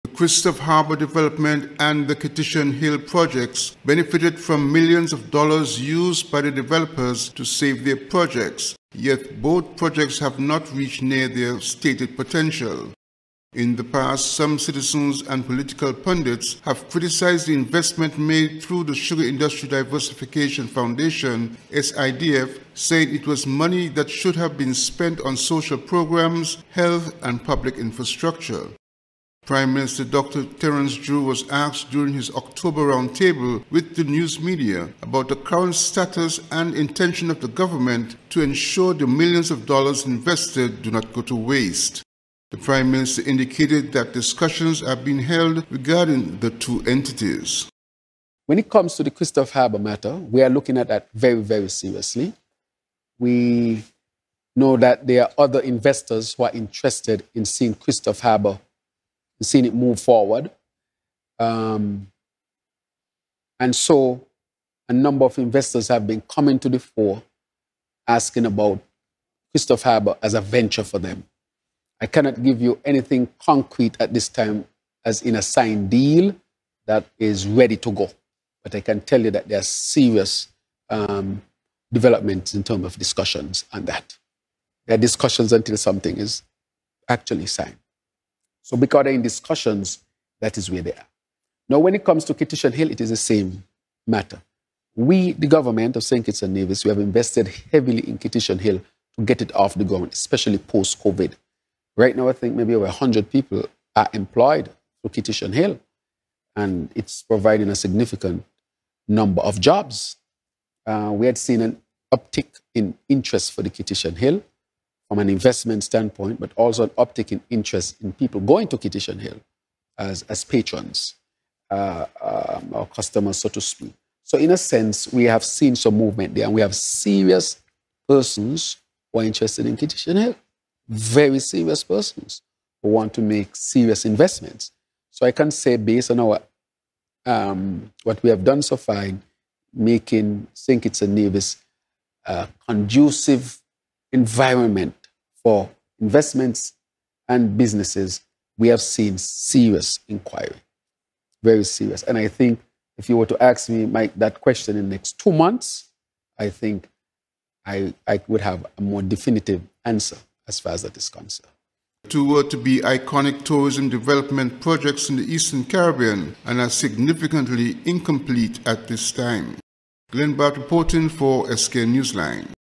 PM DREW SAYS DISCUSSIONS ON GOING FOR STALLED DEVELOPMENTS
The Christophe Harbour Development and the Kittitian Hill projects benefitted from millions of dollars used by the developers to save their projects, yet both projects have not reached near their stated potential.